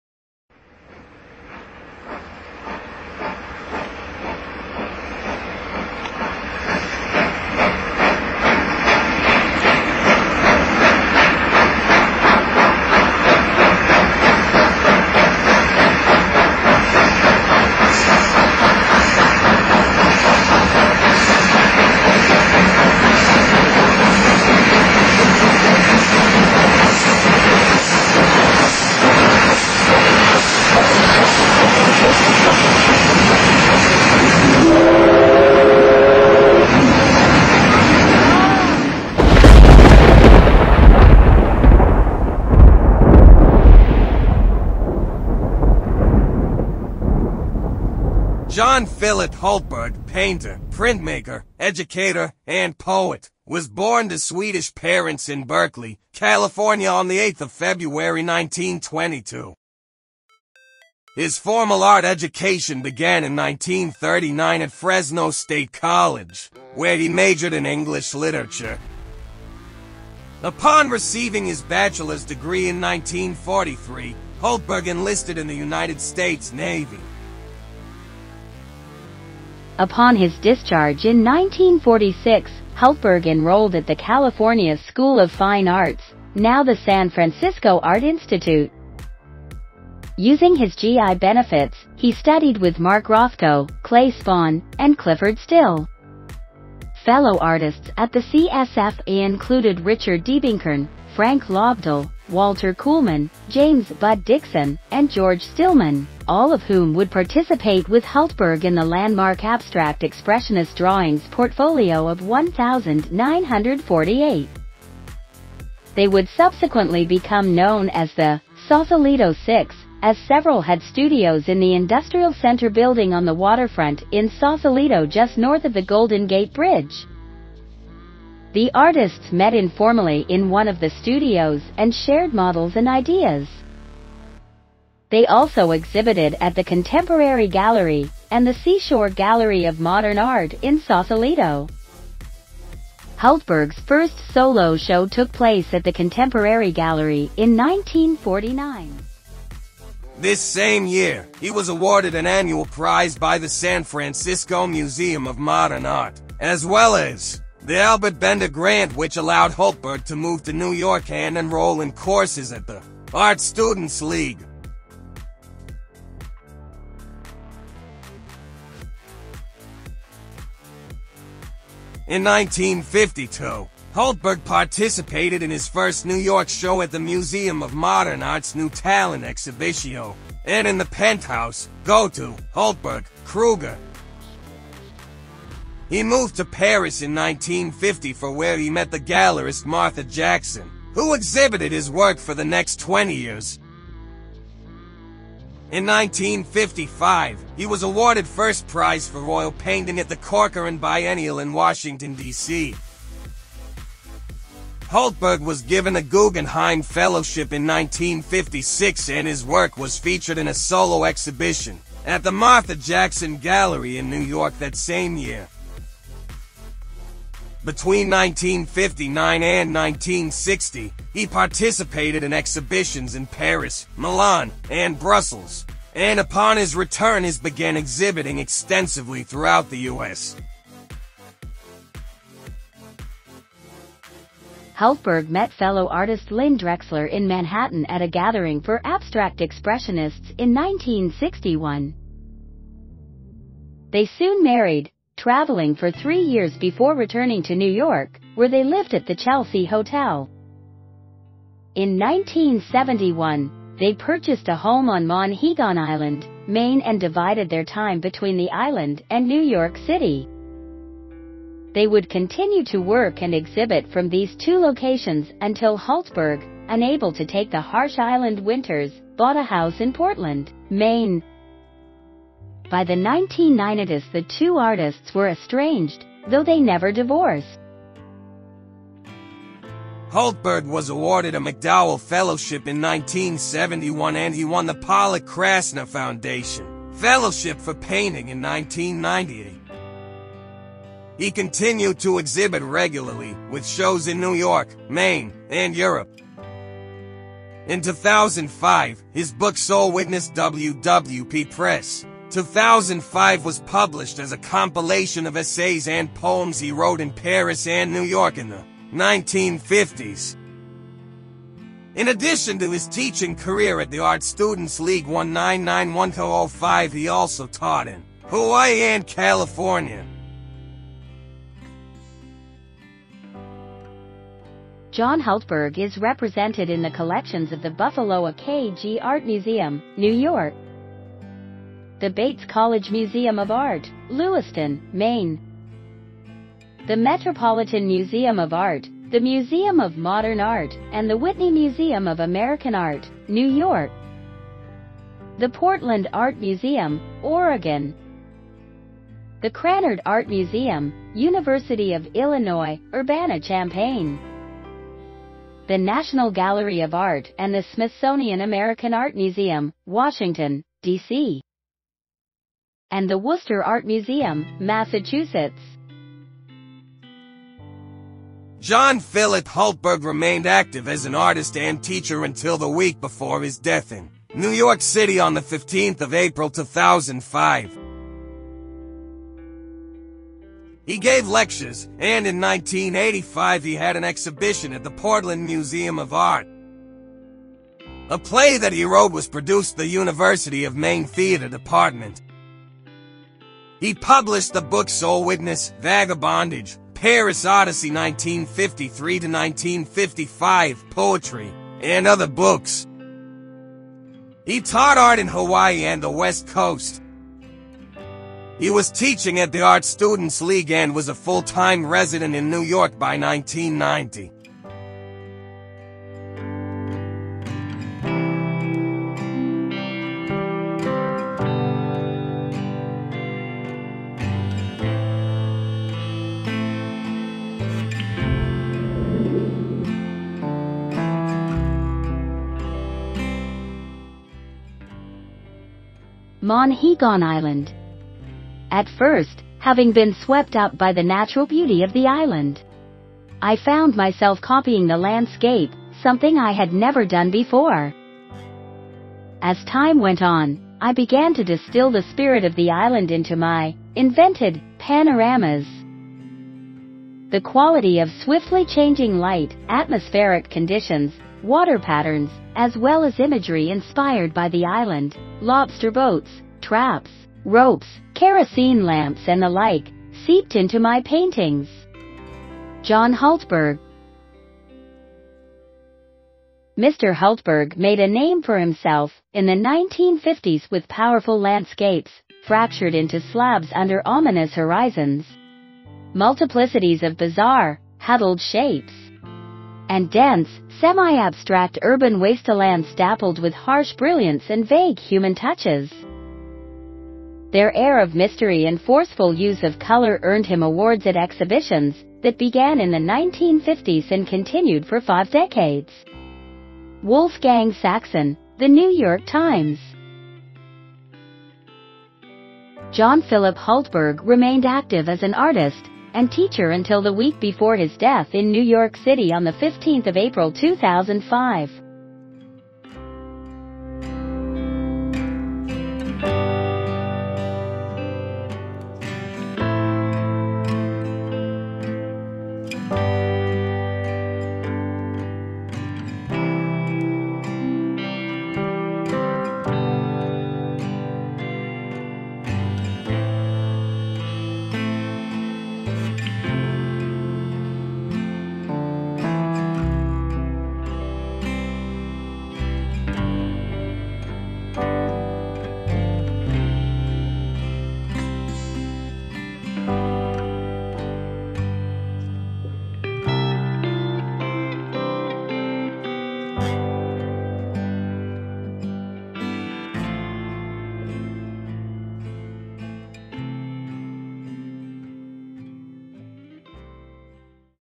John Hultberg, 1922-2005. Painter, printmaker, educator, and poet. American abstract expressionist.
John Phillip Hultberg, painter, printmaker, educator, and poet, was born to Swedish parents in Berkeley, California on 8 February 1922. His formal art education began in 1939 at Fresno State College where he majored in English literature. Upon receiving his bachelor’s degree in 1943, Hultberg enlisted in the United States Navy.
John Hultberg is represented in the collections of the Buffalo AKG Art Museum, New York; the Bates College Museum of Art, Lewiston, Maine; the Metropolitan Museum of Art, the Museum of Modern Art, and the Whitney Museum of American Art, New York; the Portland Art Museum, Oregon; the Krannert Art Museum, University of Illinois, Urbana-Champaign; the National Gallery of Art and the Smithsonian American Art Museum, Washington, D.C., and the Worcester Art Museum, Massachusetts.
 He studied with Mark Rothko, Clay Spohn, and Clyfford Still.